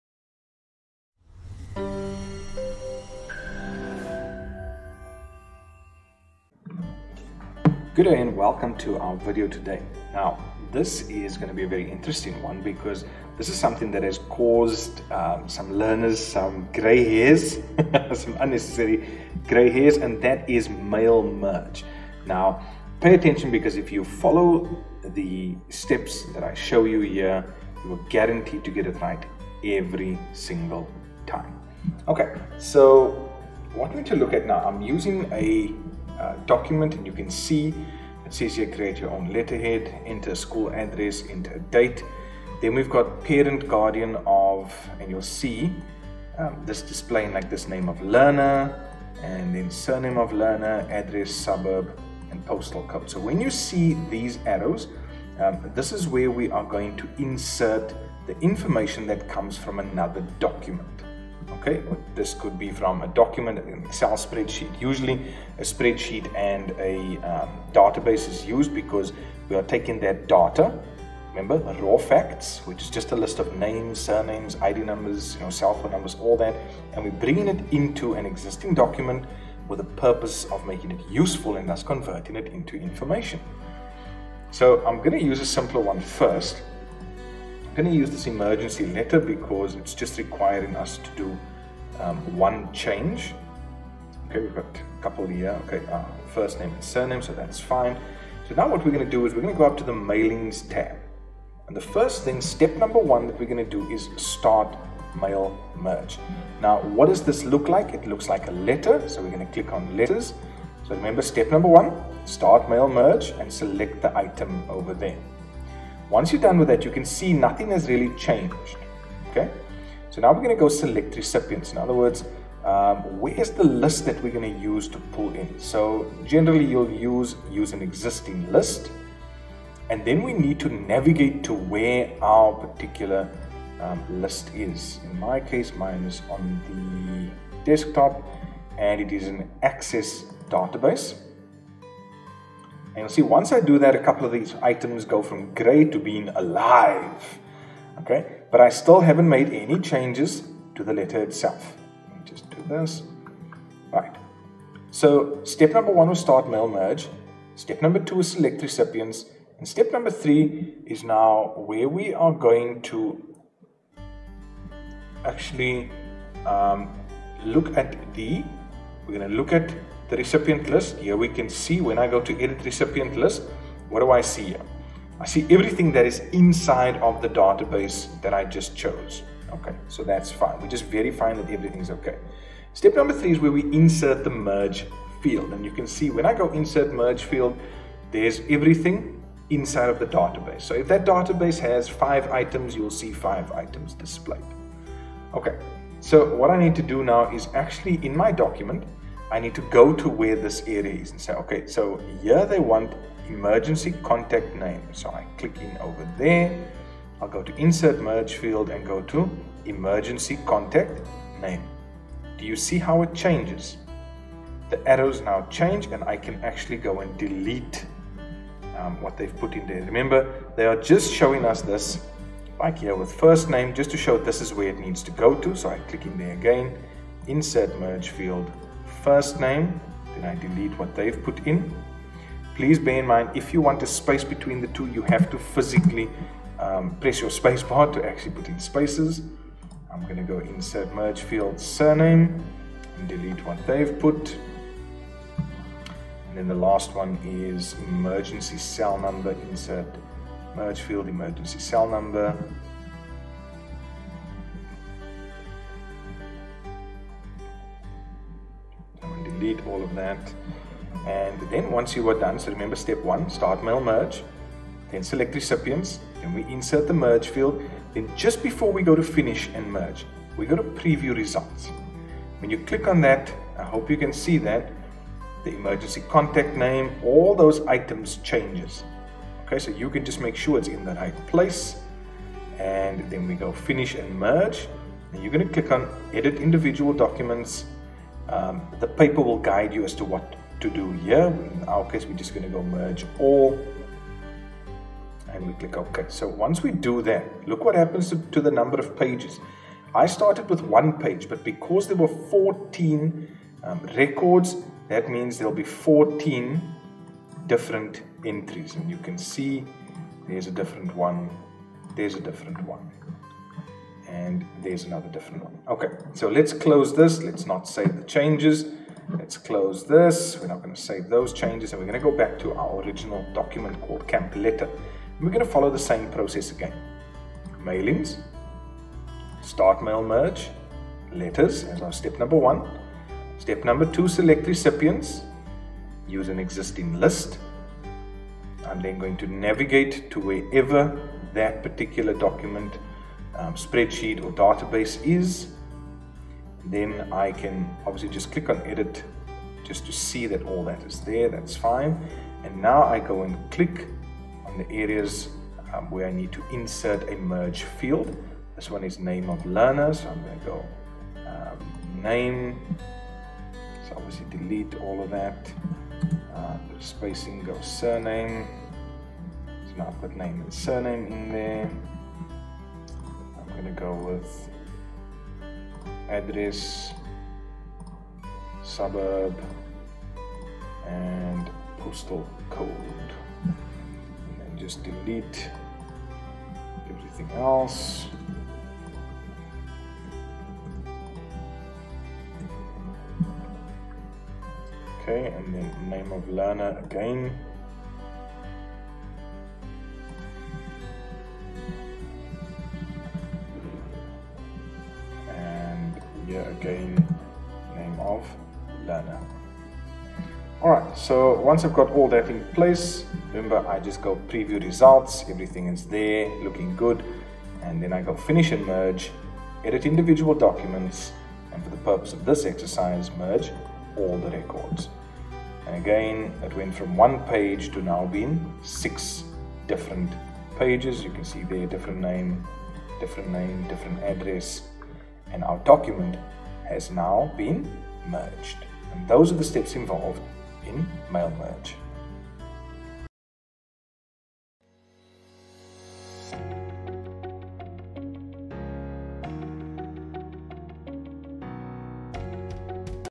Mail Merge
Let's go through the 4 Basic Steps involved in the Mail Merge process PLUS using the Editing Recipients Feature